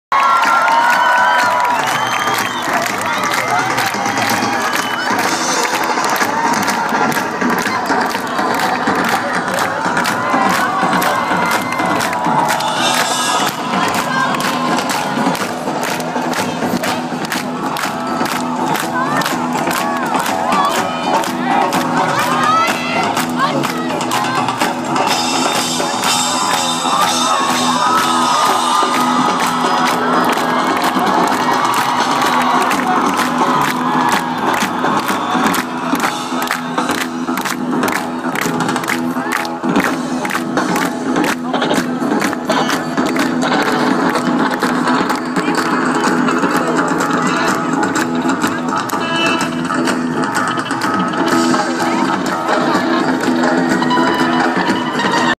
The Red Hot Chili Peppers coming on stage in Turkey for the first time ever Sept 8th 2012
The Red Hot Chili Peppers coming on stage in Turkey for the first time ever September 8th 2012 - Istanbul, Turkey